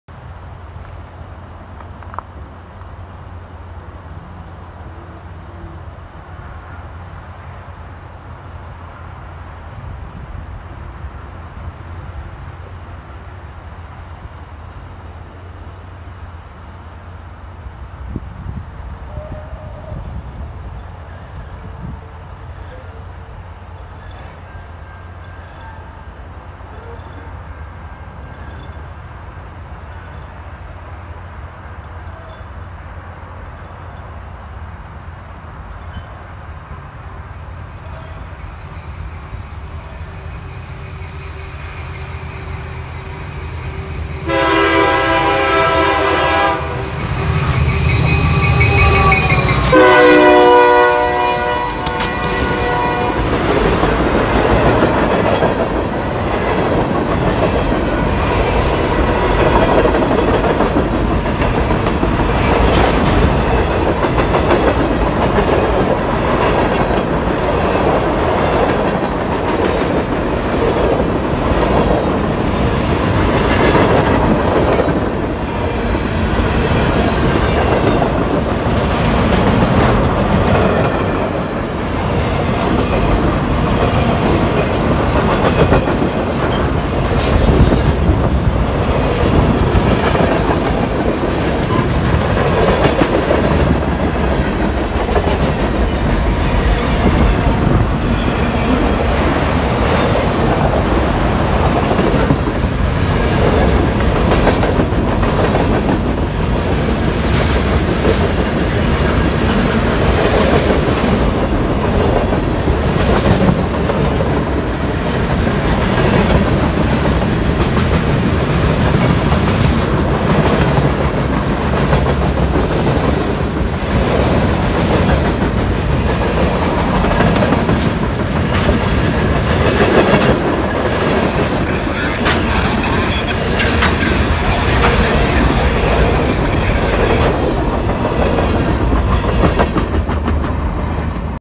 NS 331-22 w/ GMTX SD60 #9014, NS SD70M #2599, and GECX C41-8W #9539 @ South Rockwood, MI 10/22/18
Shot on the NS Detroit line in South Rockwood, MI.